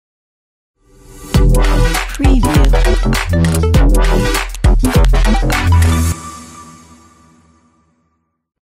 Glitch Intro After Effects Templates
Glitch Intro is an intense-looking and dramatically animated After Effects template. It uses a stunning combination of effects to reveal and enhance your media. It contains 4 text placeholders, 1 media placeholder and 1 logo placeholder. A powerful introduction to presentations, slideshows, TV shows, commercials, promotions and event videos. Impress your audience with this action-packed and energetically animated AE template. No plugins required.